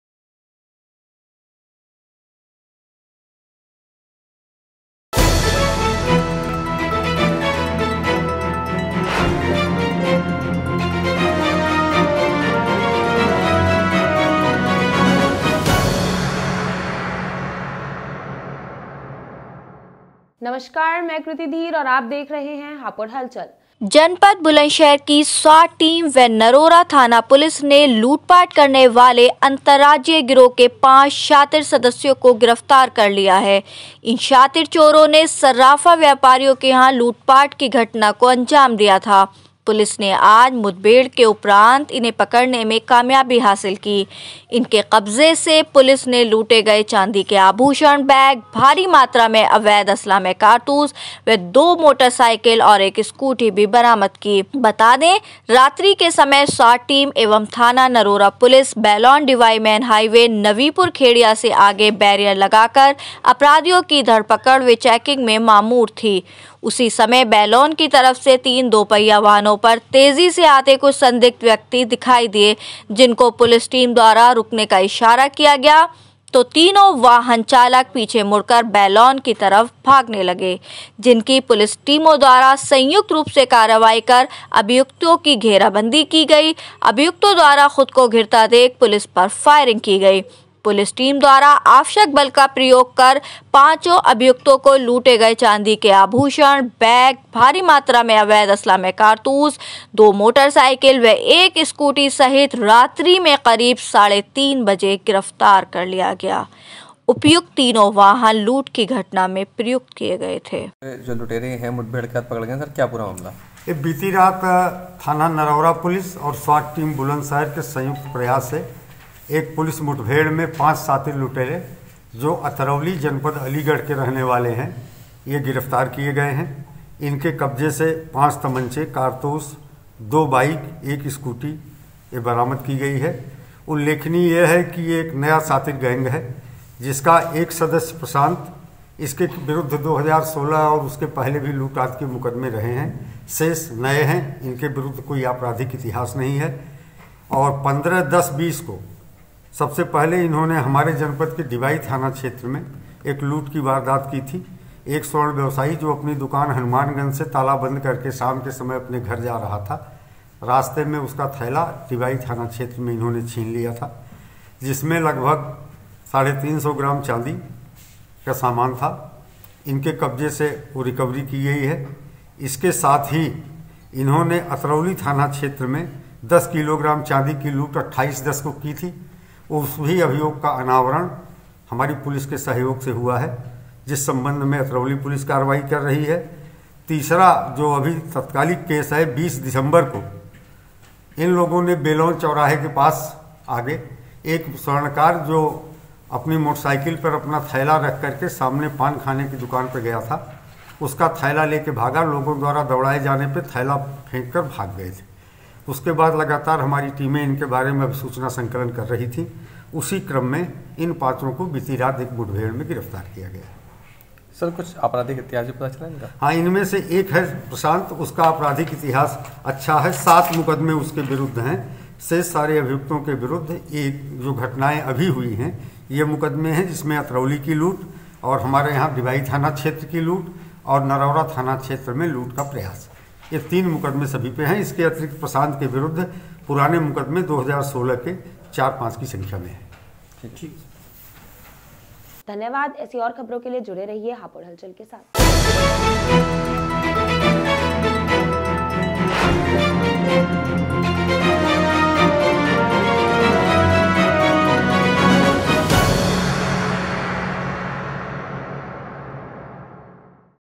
लूट की घटनाओं को अंजाम देने वाले पांच लूटेरे गिरफ्तार
स्वाट टीम व नरौरा थाना पुलिस ने मुठभेड़ के उपरांत सर्राफा व्यापारियों से लूटपाट करने वाले अंतरराज्यीय गिरोह के 5 शातिर सदस्य किए गिरफ्तार

कब्जे से लूटे गए चांदी के आभूषण, बैग, भारी मात्रा में अवैध असलाह मय कारतूस व 2 मोटरसाइकिल और 1 स्कूटी बरामद।

रात्रि के समय स्वाट टीम एवं थाना नरौरा पुलिस बैलौन--डिवाई मेन हाईवे नवीपुर खेड़िया से आगे बैरियर लगाकर अपराधियों की धरपड़क व चैकिंग में मामूर थे।

 उसी समय बैलौन की तरफ से 3 दुपहिया वाहनों पर तेजी से आते कुछ संदिग्ध व्यक्ति दिखाई दिए, जिनको पुलिस टीम द्वारा रुकने का इशारा किया गया तो तीनों वाहन चालक पीछे मुड़कर बैलौन की तरफ भागने लगे।

 दोनों पुलिस टीम द्वारा संयुक्त रुप से से कार्रवाई कर अभियुक्तों को घेरा बंदी की गई जिस पर अभियुक्तों द्वारा अपने को घिरता देख पुलिस पर जान से मारने की नियत से फायर किए गए।

 पुलिस टीम द्वारा आवश्यक बल प्रयोग कर सभी पांचों अभियुक्तों को लूटे गए चांदी के आभूषण, बैग, भारी मात्रा में अवैध असलाह मय कारतूस व 2 मोटरसाइकिल/ 1 स्कूटी सहित रात्रि में लगभग 3:30 बजे गिरफ्तार करने कर लिया गज्ञया।

बरामदगी:
1--500 ग्राम आभूषण सफेद धातु।
2--एक बैग।
3-- 4 तमंचा 315 बोर मय 6 जिंदा व 4 खोका कारतूस
4--एक अपाचे मोटरसाइकिल बिना नंबर
5--एक पल्सर मोटरसाइकिल यUP 81AM 5748
6--एक स्कूटी जूपिटर UP 81 BK 2760!
उपयुक्त तीनों वाहन  लूट की घटना में प्रयुक्त किए गए थे।